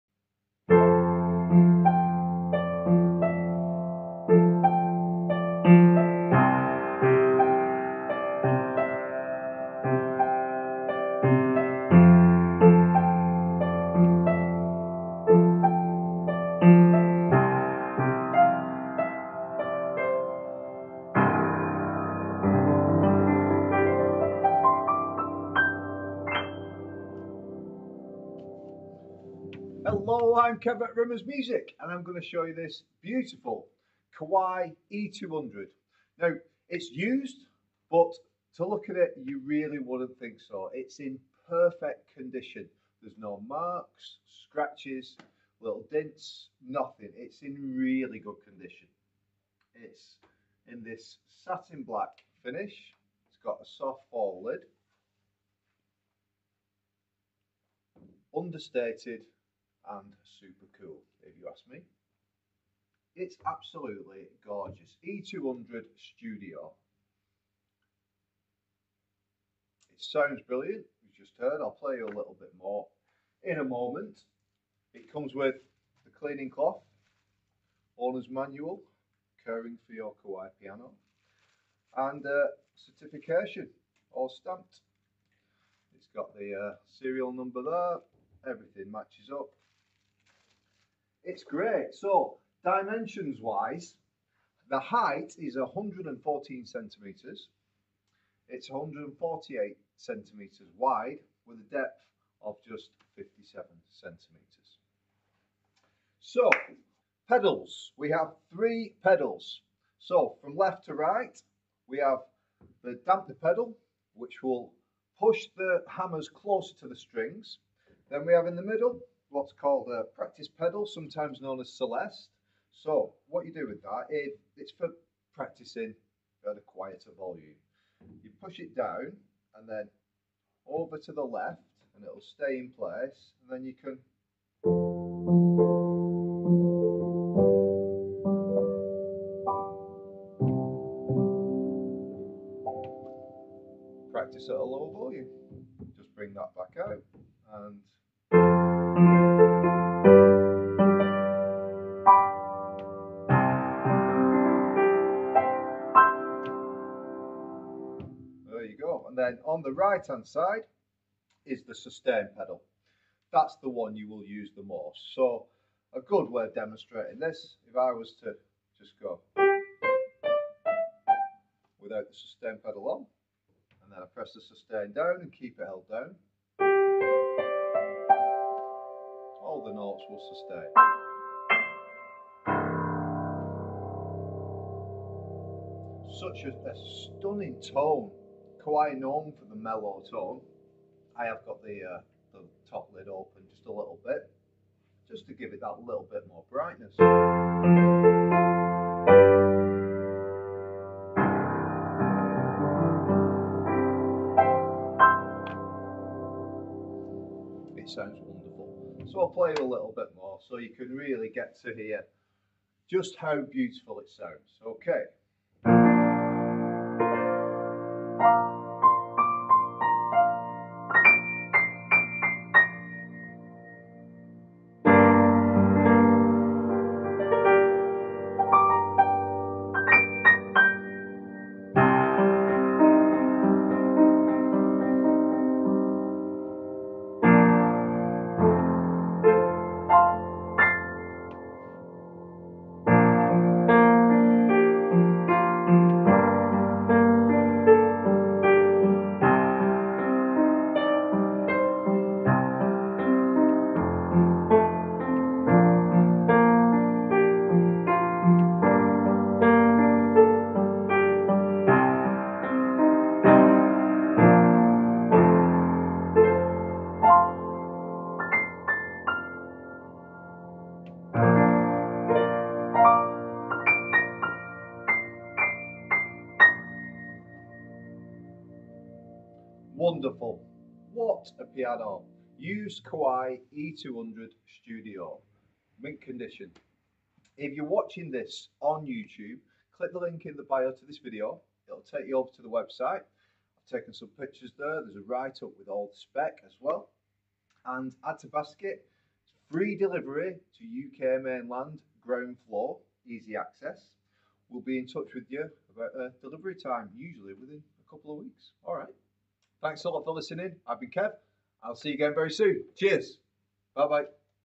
Used Kawai E200 Studio Satin Black Upright Acoustic Piano - Rimmers Music
Here we have Kev demonstrating the beautiful tone of this used, Kawai E-200 Studio, in satin black.

This Kawai E200 has had one previous owner, in a smoke free environment, has been regularly tuned and extremely well looked after. There are no dints, marks or scratches anywhere on this piano, it is like new; mint condition. Known for their mellow sound, this model is a great example of Kawai’s balanced tonal output. It is great value for money and has a lovely, responsive action/touch that will suit beginners and pro’s alike.